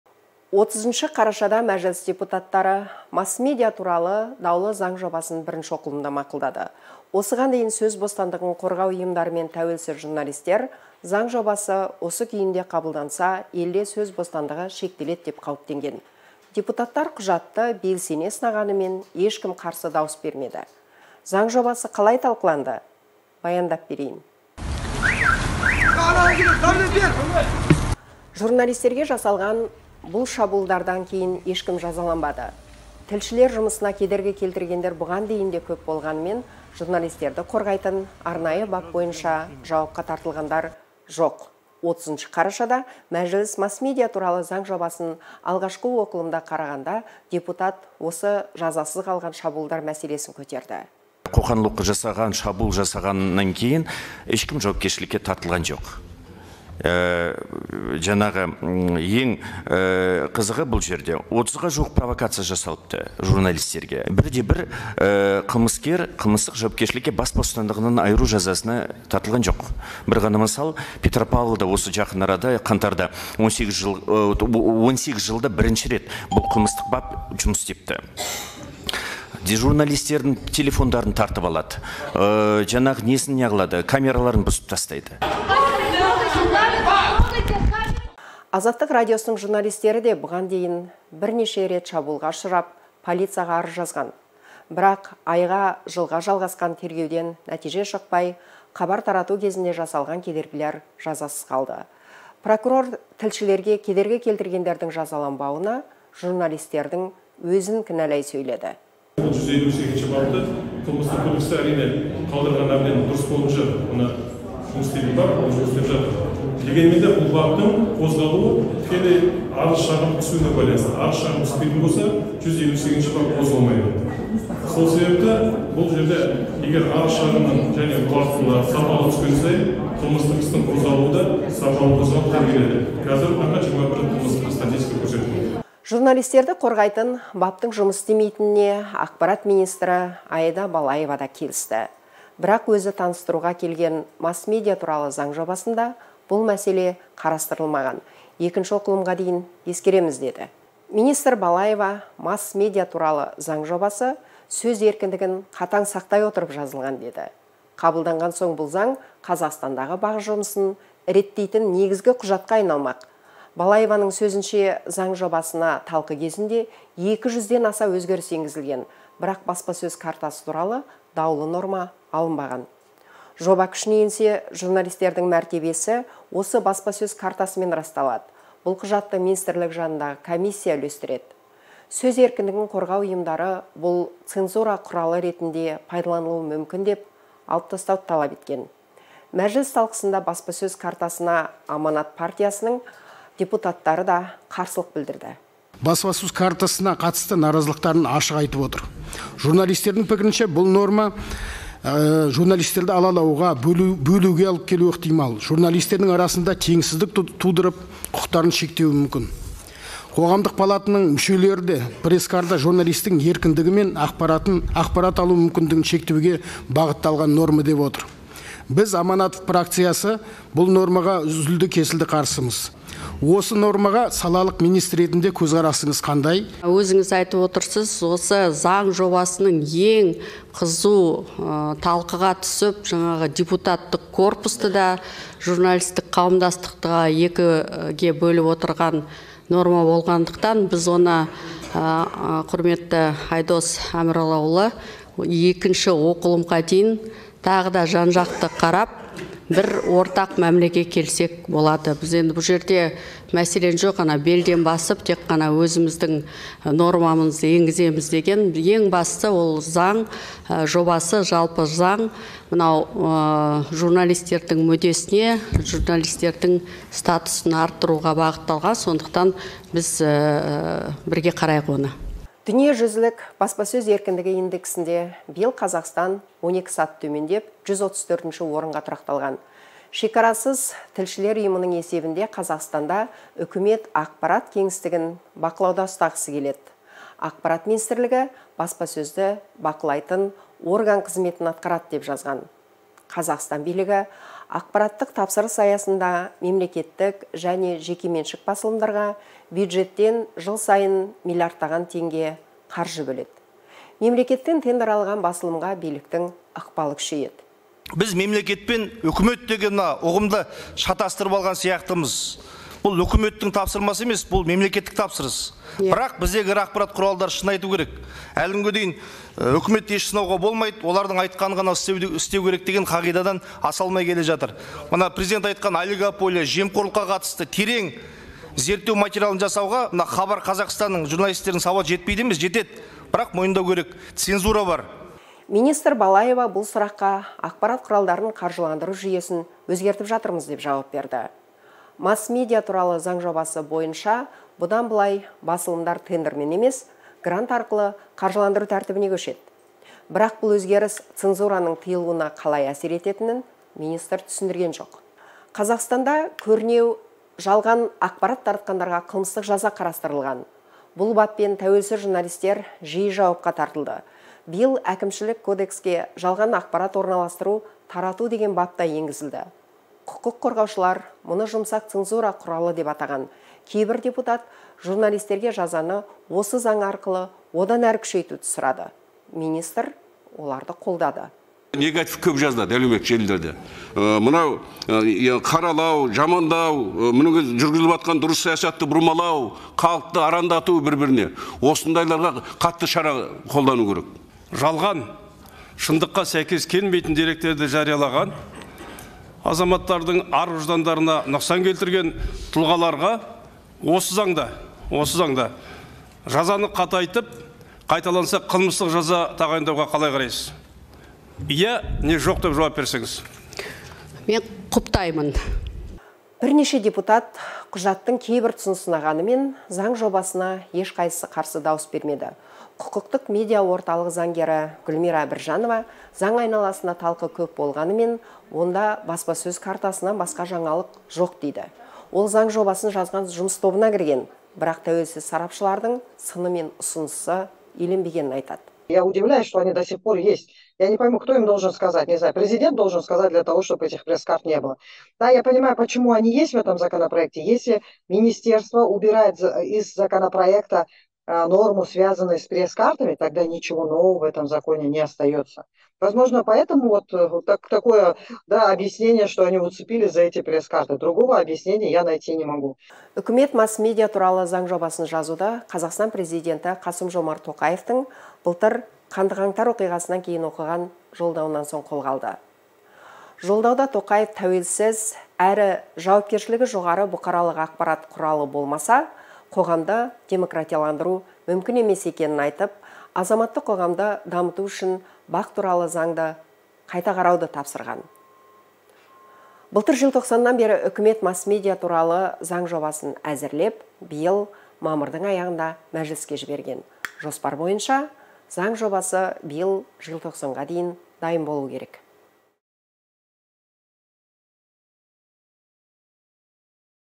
Депутаттар даулы заң жобасын қалай мақұлдады?
30 қарашада мәжіліс депутаттары "Масс-медиа туралы" даулы заң жобасын бірінші оқылымда мақұлдады. Осыған дейін сөз бостандығын қорғау ұйымдары мен тәуелсіз журналистер "заң жобасы осы күйінде қабылданса, елде сөз бостандығы шектеледі" деп қауіптенген. Құжат бойынша қызу талқы жүріп, бірнеше сын айтылғанымен, ешбір депутат жобаға қарсы дауыс бермеді.